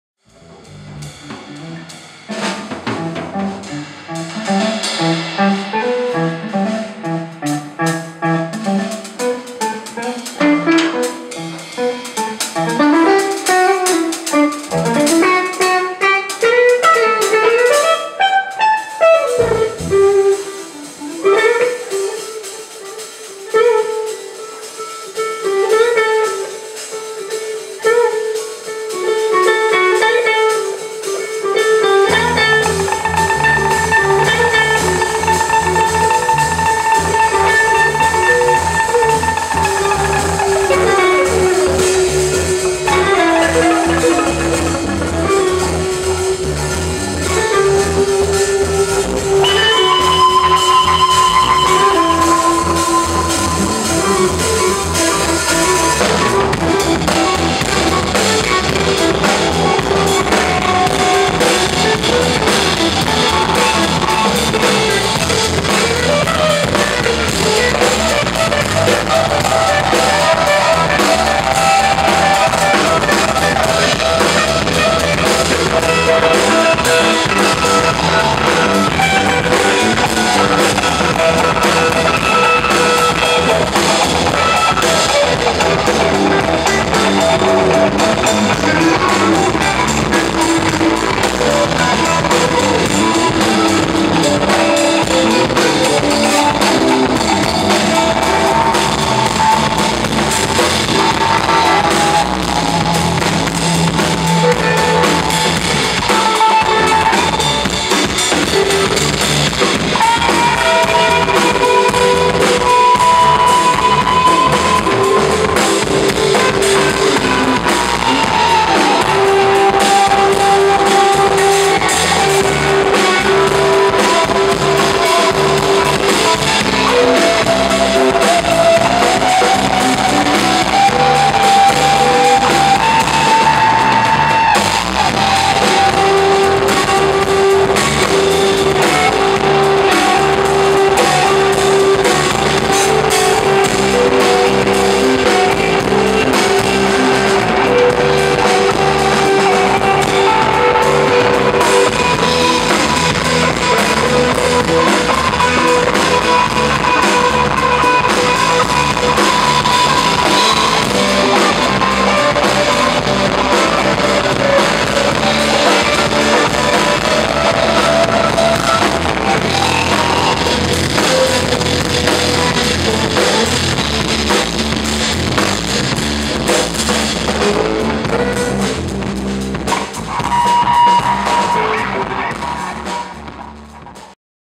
SOLAR MUSIC - фрагменты
27 сентября 2013 года
Евгений Барташевич, Павел Тимонин, Кирилл Трошин, Егор Тупейко